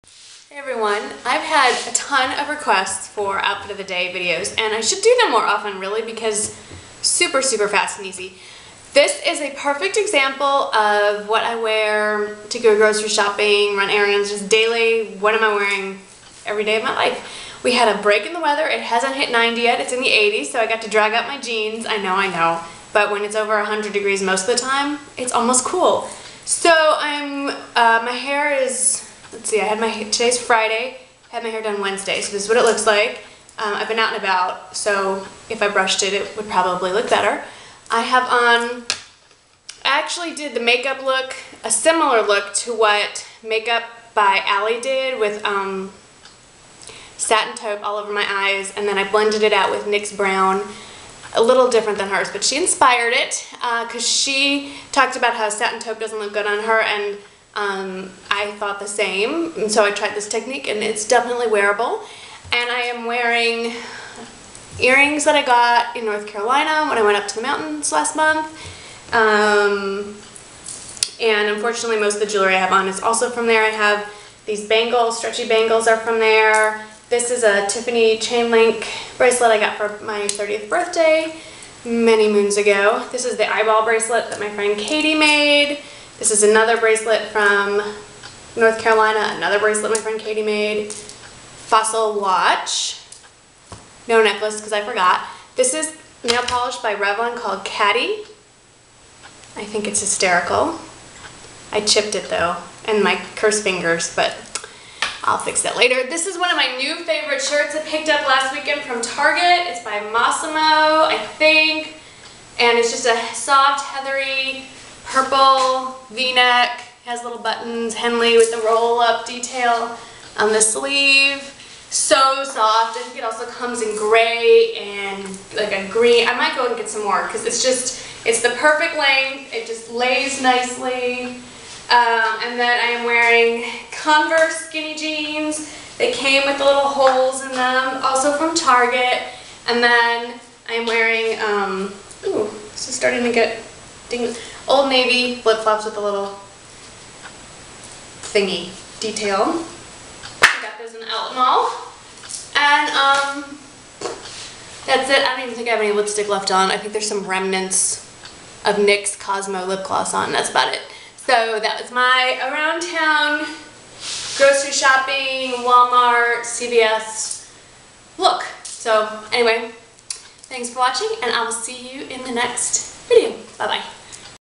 Grocery Shopping OOTD
BEFORE YOU ASK ME A QUESTION, READ THIS FIRST!!!

What I'm Wearing:
Eyes: MAC Satin Taupe on the lid blended out with NYX Brown
Lips: NYX Cosmo
Nails: Revlon Catty
Shirt: Target
Jeans: Converse for Target
Watch: Fossil
Shoes: Old Navy